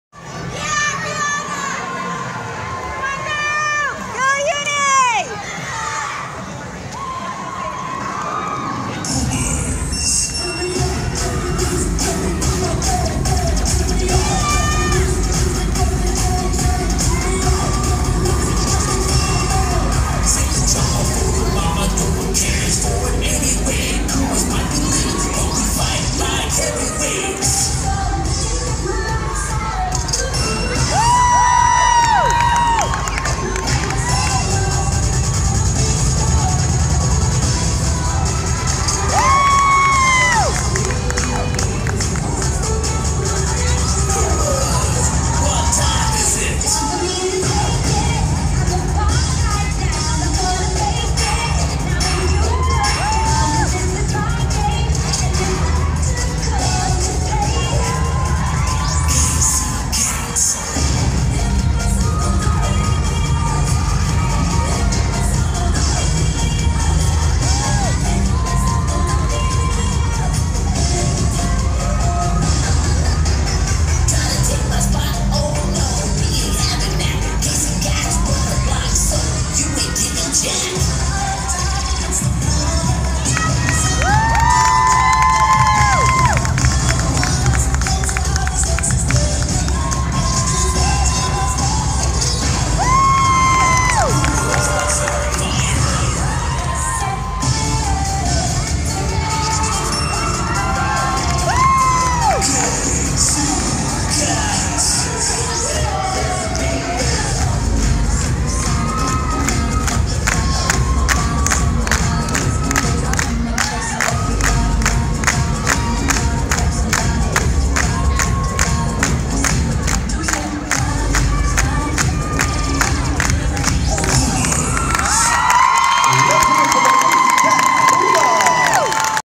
KC Cats Pumas at URI RI Rumble Championship
KC Cats Pumas routine at the RI Rumble Championship on January 6, 2018 at URI Ryan Center.